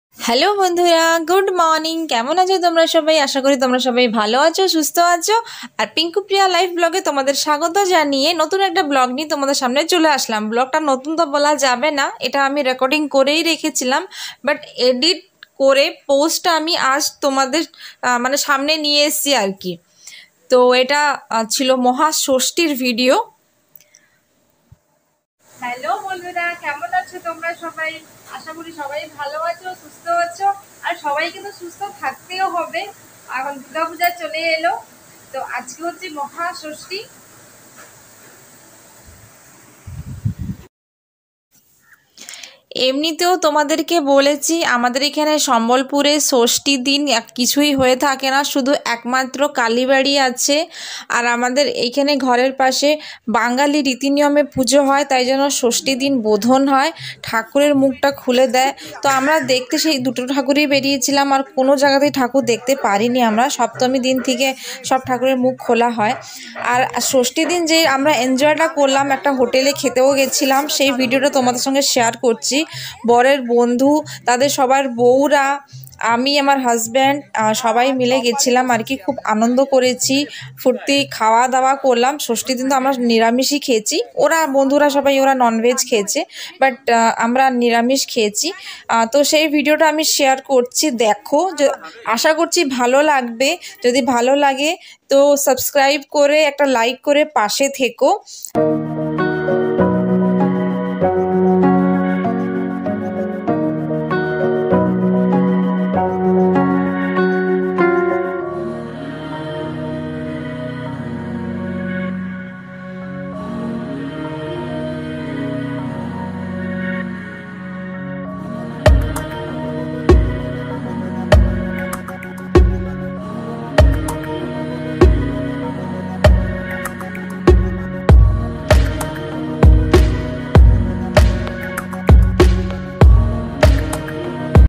শুভ মহা ষষ্ঠী ২০২২/late post/বন্ধু দের সাতে ঘোরা খাওয়া দাওয়া আনন্দ হলো/Durga puja Status
শুভ মহা ষষ্ঠী ২০২২/late post/বন্ধু দের সাতে ঘোরা খাওয়া দাওয়া আনন্দ হলো/Durga puja Status||Bengali vlog @Sumani683

bondhura video ti Tomader songe share korte onek ta e deri kore diechi tar jonno khoma prarthi 🙏 tao request roilo video ti puro dekhar bhalo lagle ekta like ar chennal ti subscribe kore passe theko

Tomader sobar jonno Bhalobasa Roilo ❤️